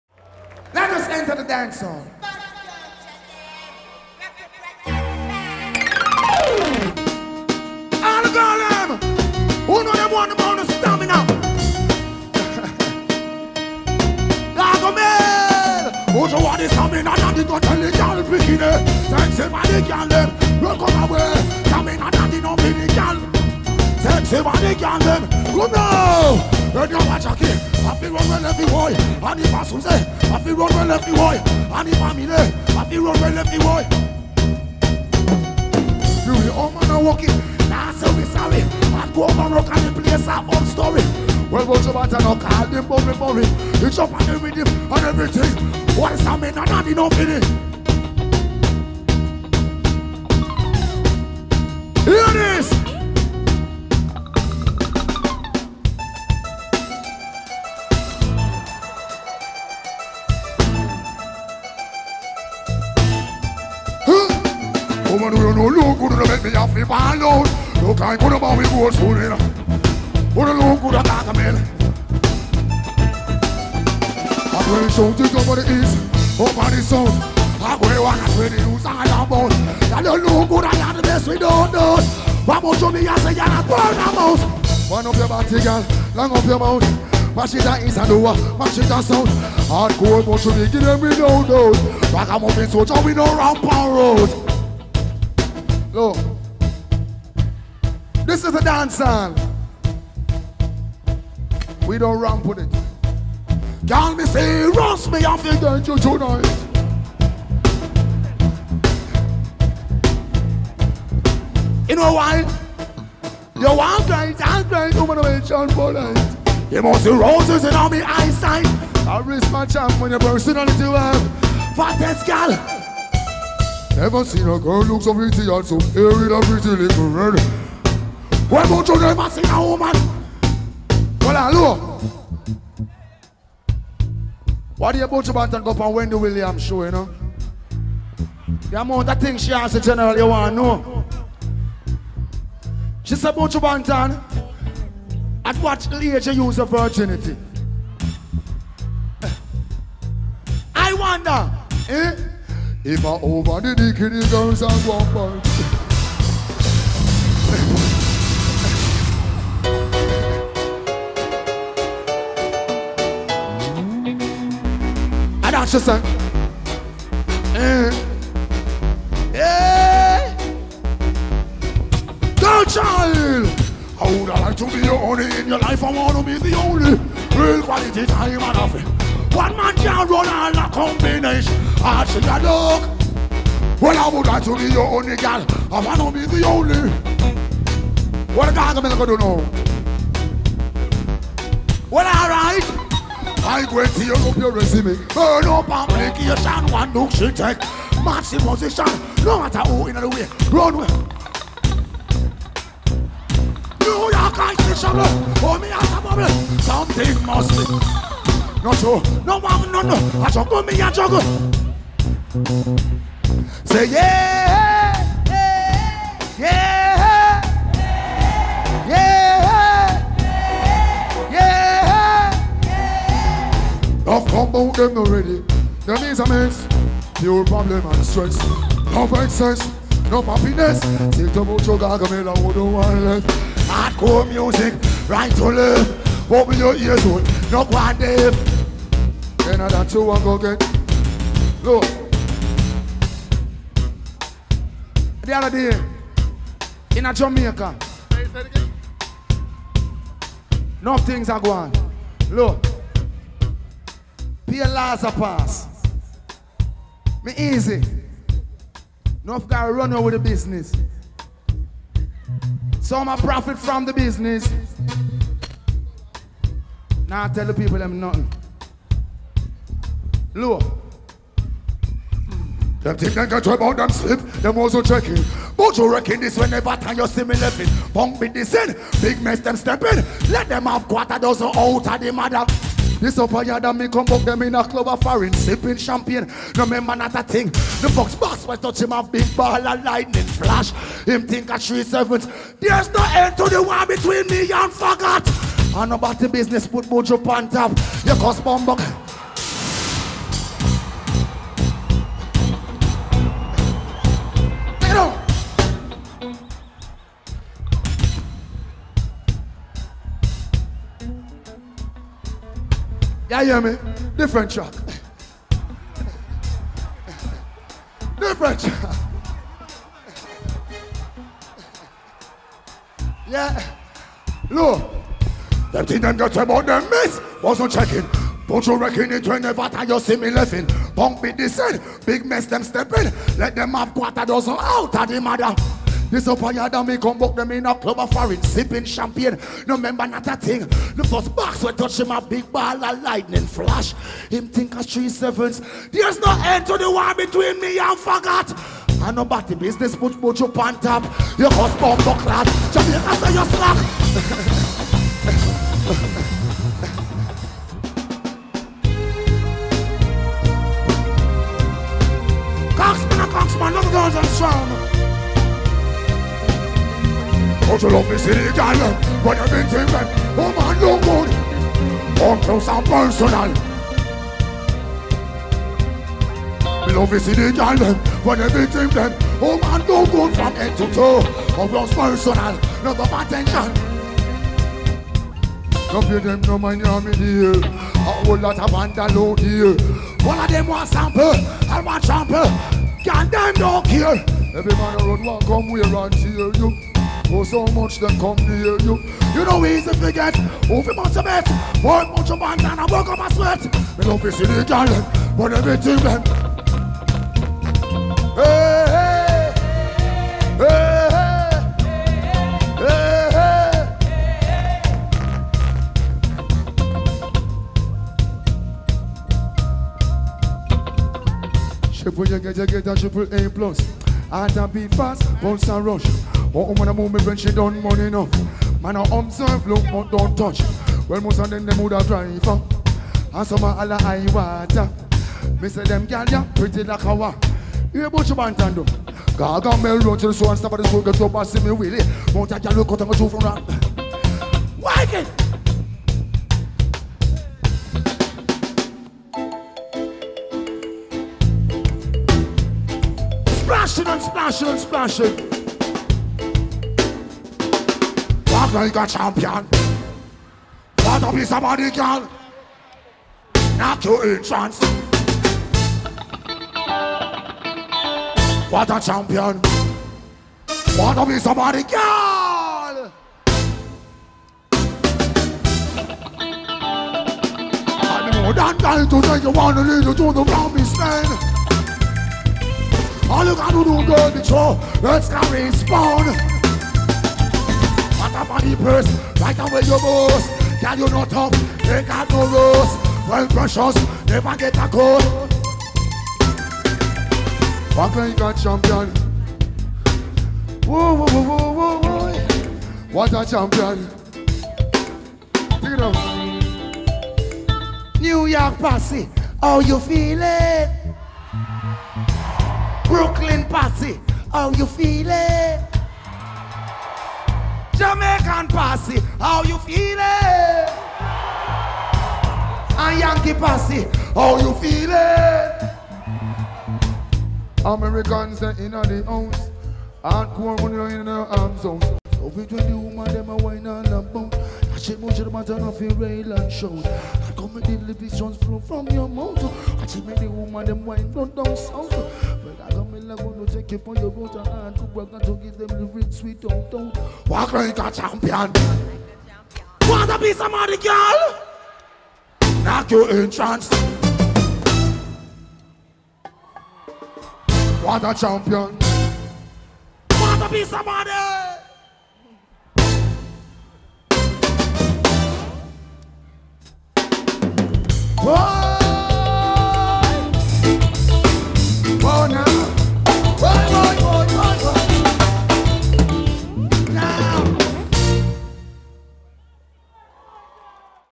Buju Banton LIVE in NYC | Part 3
This archived footage features Buju Banton performing LIVE in NYC with a full band + background singers. Support reggae music!